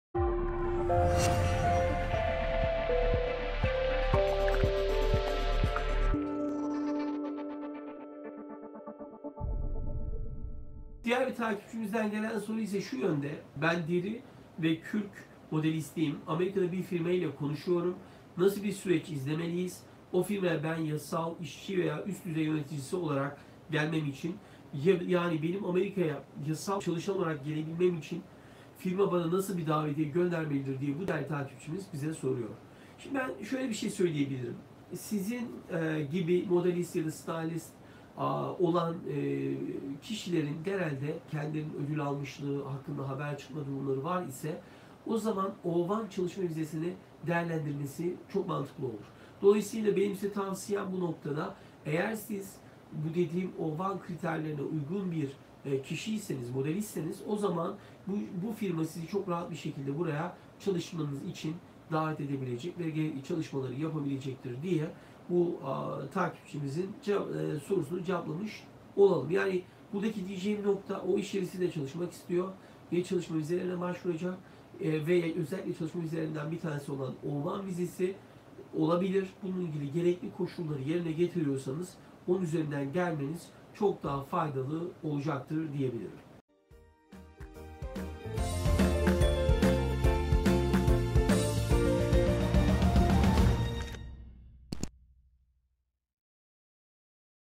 AMERİKA'YA YASAL ÇALIŞAN OLARAK GELMENİN YOLU NEDİR ?
Soru: Ben deri ve kürk modelistiyim. Amerika’da bir firma ile konuşuyorum, nasıl bir süreç izlemeliyiz, o firmaya ben yasal işçi veya üst düzey yönetici olarak gelmem için. Yani benim Amerika’ya yasal çalışan olarak gelebilmem için firma bana nasıl bir davetiye göndermelidir ?